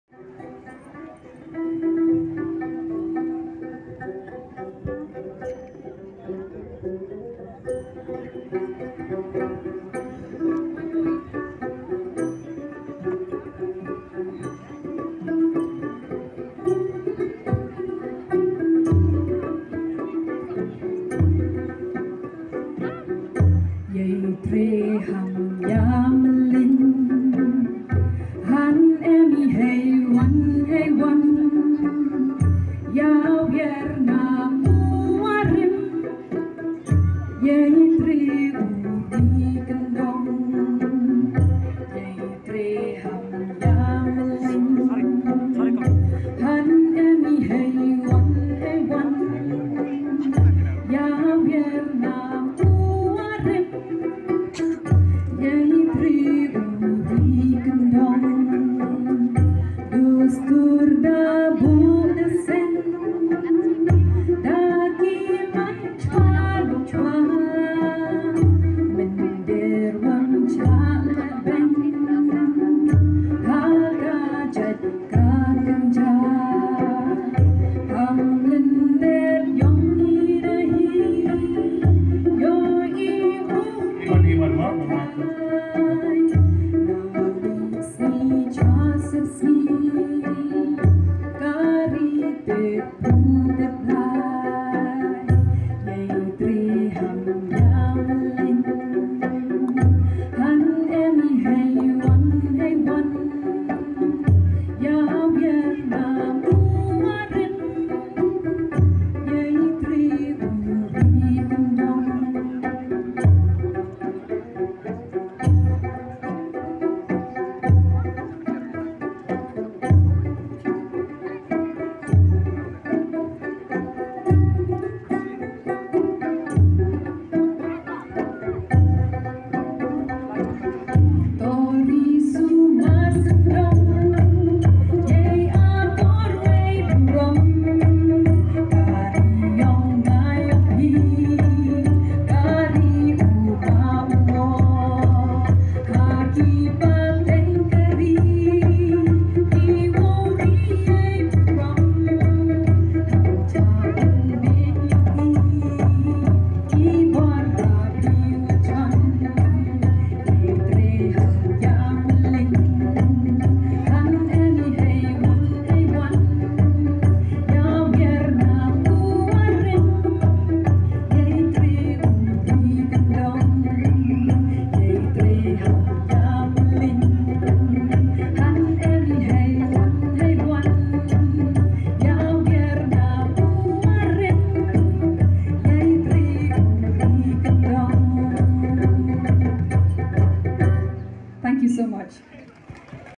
Chad sukra jowai
Traditional dance in Jowai West jaintia hills district 2022